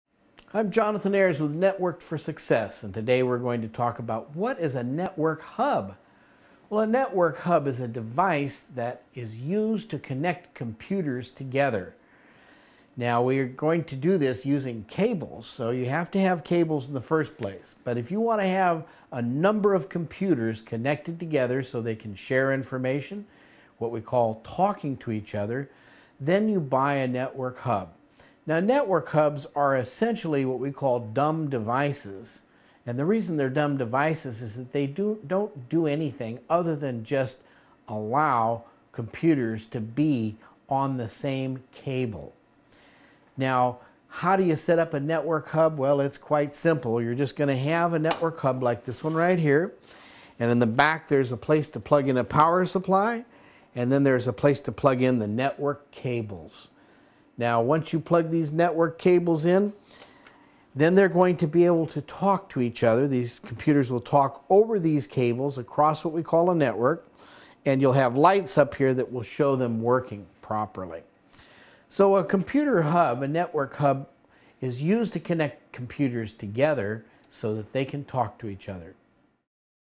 Computer Basics : What Is a Network Hub?
A network hub is used to connect computers together so that they can communicate with each other over the same cable. Set up a network hub by plugging in network cables from different computers using help from a certified computer technician in this free video on computer networks.

Expert: Jonathan Ayres
Bio: Jonathan Ayres has more than 25 years of computer industry experience with all types of computer hardware and operating systems.